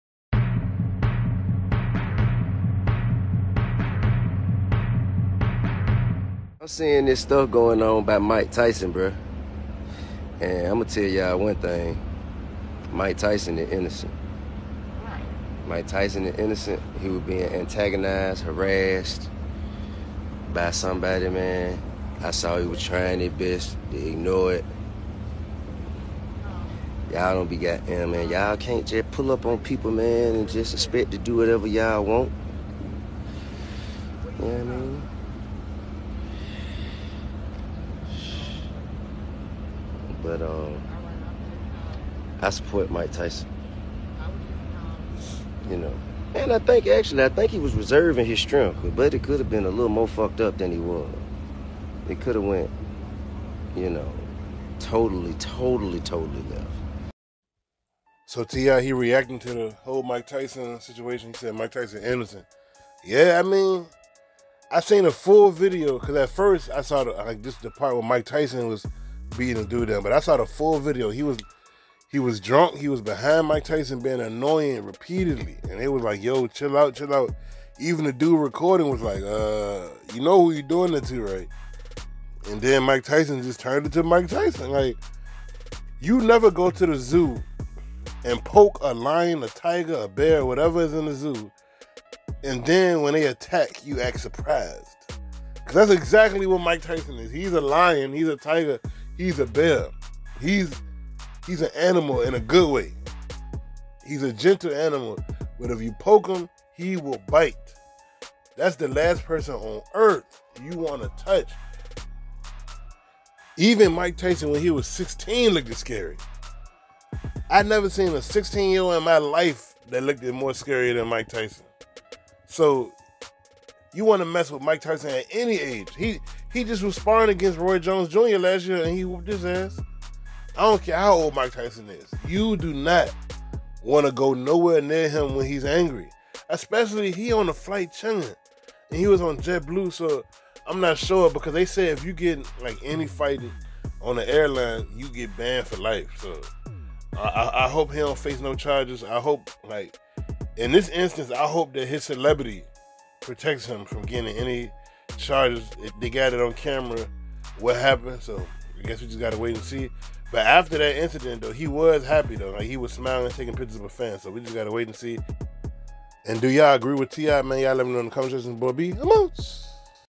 T.I. Reacts & Supports Mike Tyson Punching Out Fan! "It Could've Went Really Left"
do u agree with t.I.